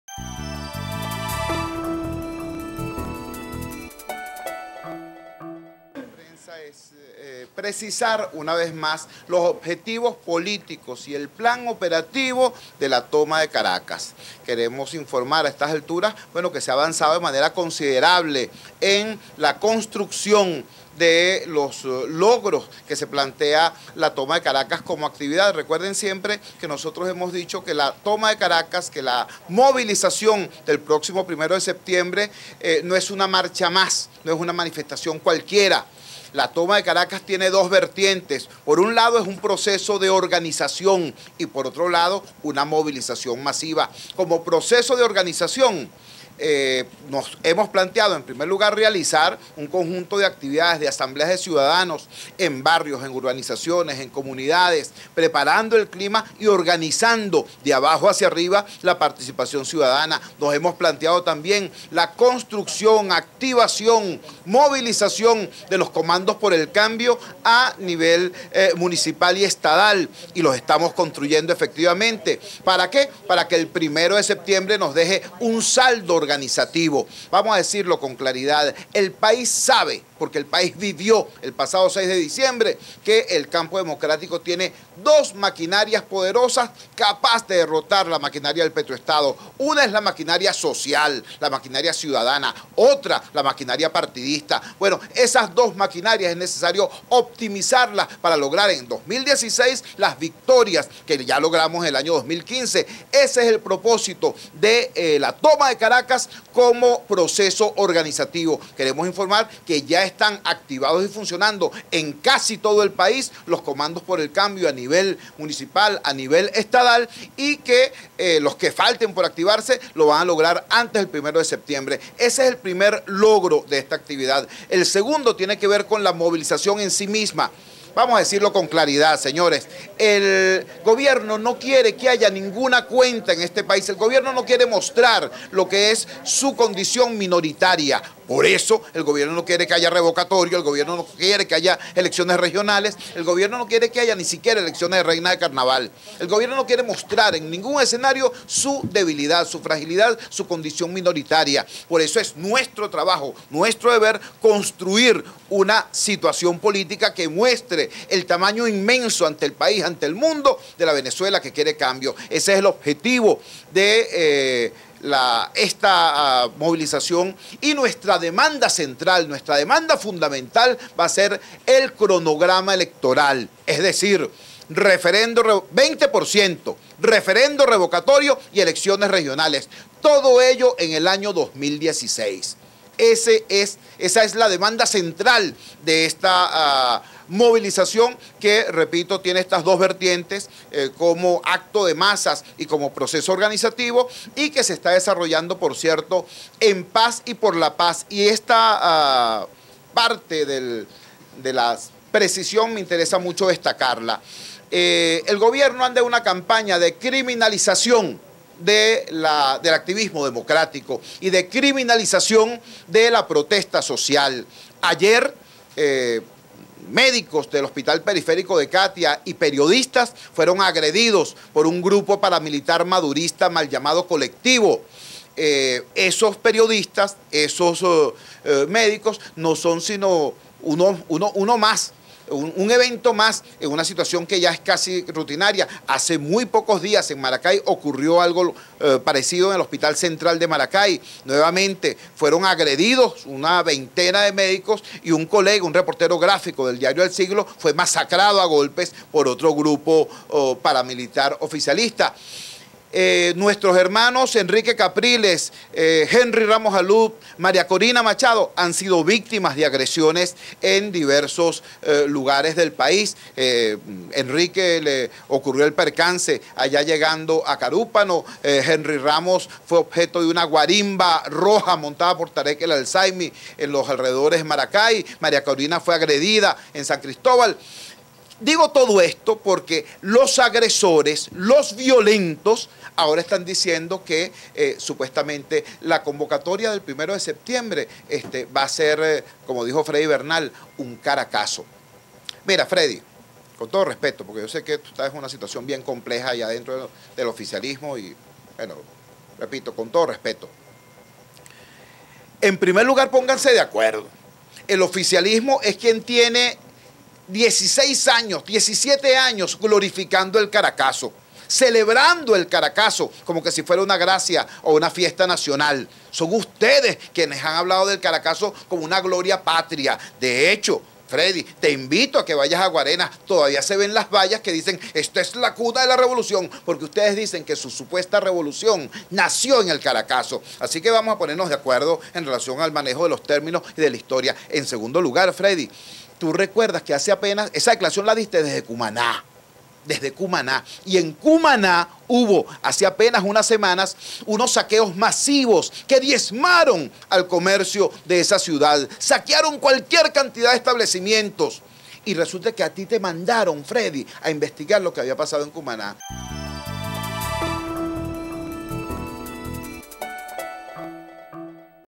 Torrealba: "Toma de Caracas" demostrará que el pueblo quiere revocatorio
El secretario de la Mesa de la Unidad Democrática (MUD), Jesús "Chuo" Torrealba, explicó que la "Toma de Caracas", a realizarse el próximo el 1 de septiembre, no será una marcha más, debido a que "ese día se movilizará masivamente el pueblo por un proceso de cambio".